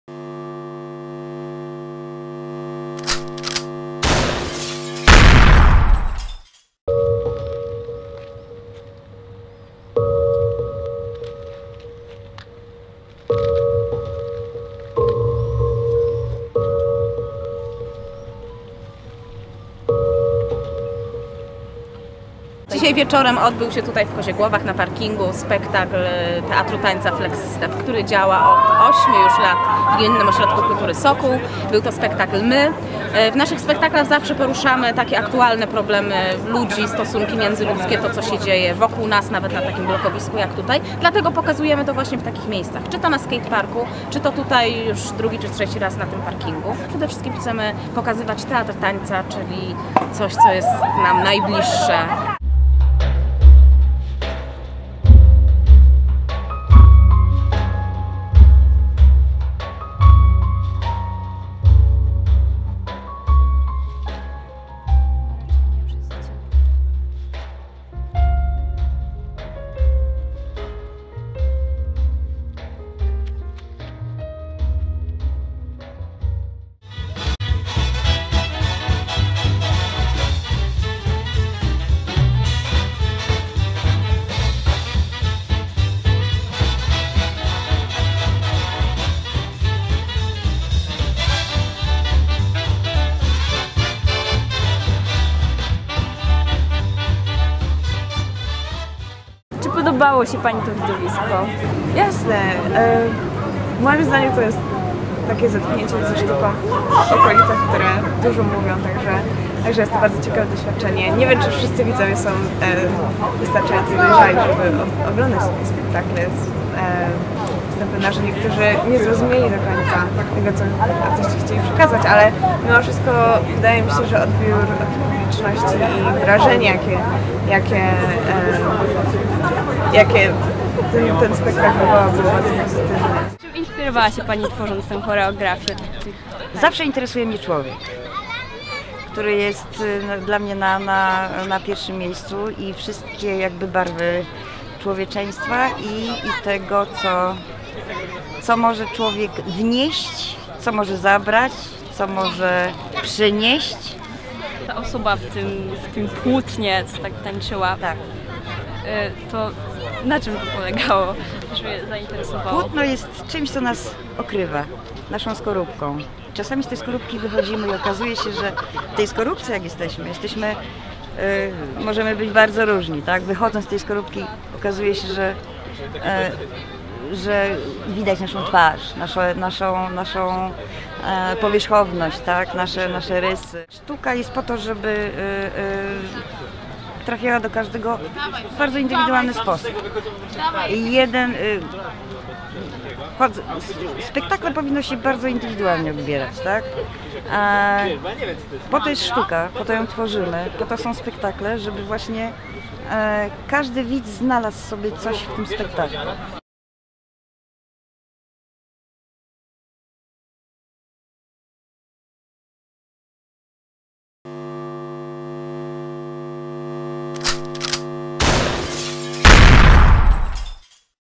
CoolturalnaTV.pl - spektakl "My" Teatru Tańca Fleks Step 29.06.2015
Kolejny reportaż przygotowany przez młodzież z Gimnazjum w Koziegłowach.